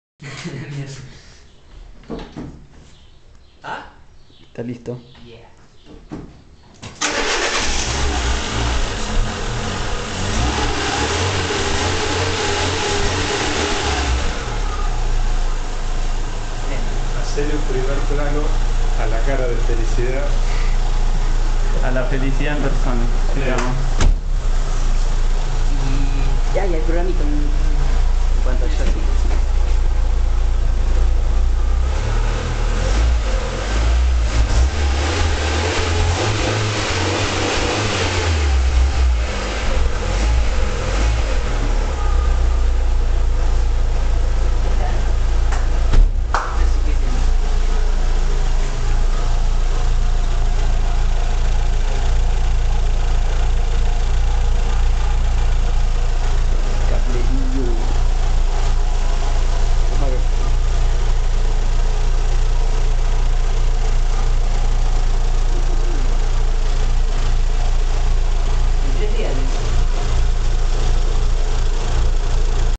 Una inyección electrónica argentina : ) -El primer arranque-
This is my ECU running on a fiat palio 1.3L 16v. Fully sequential, pic-based, and ultra cheap.
The engine started in the first attempt, just plug and play :-)

Algorithm: speed-density (no TPS in this test, thats why there isn't accel pump)
micro: PIC16F877A @ 20MHz
PC software: ECU manager v0.45 running on Debian Linux (GPL!)
I think it wasn't synchronized, but still...

Viva Argentina!